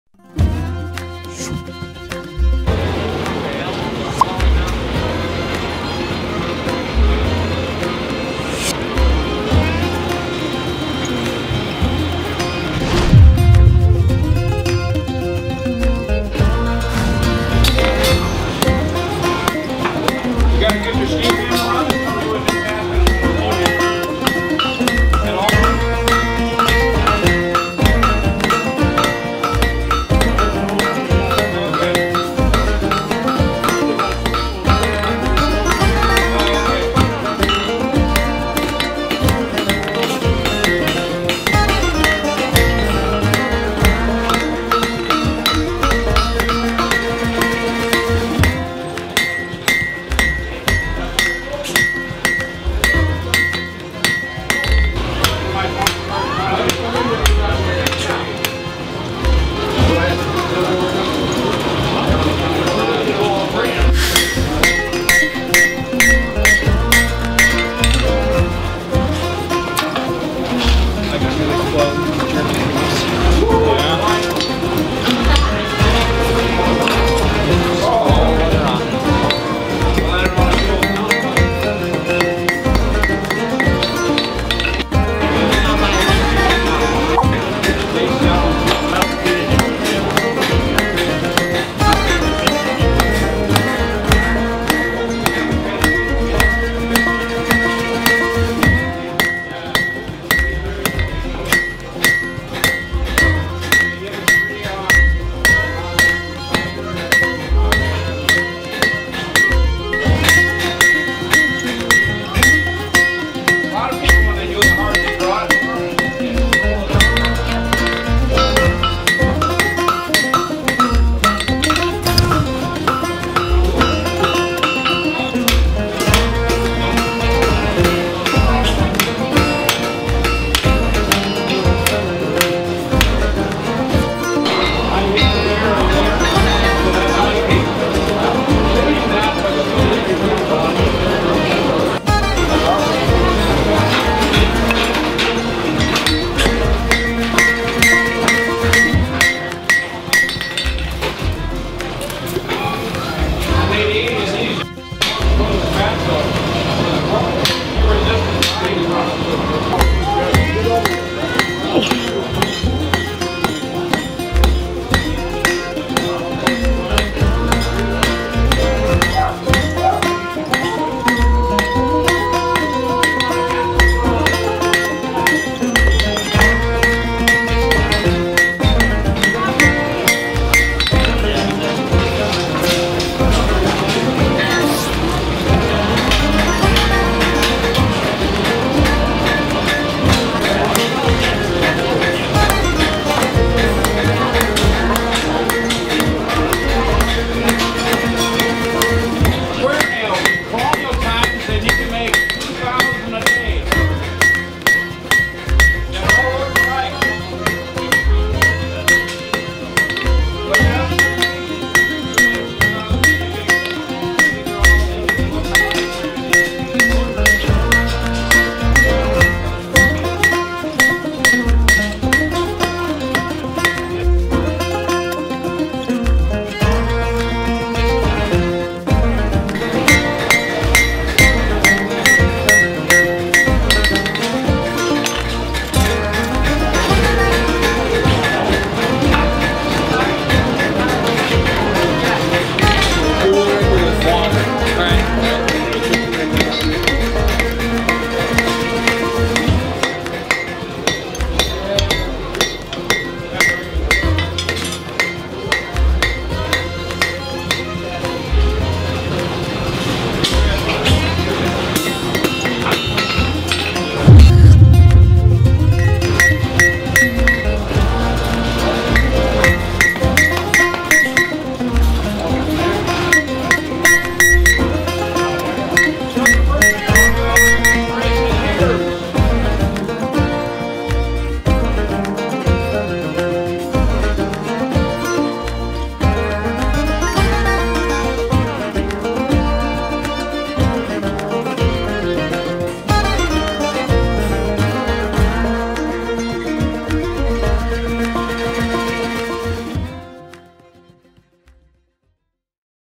Forging Nails | Rollag WMSTR
In this video Brendan forges some nails in the Rollag blacksmith shop.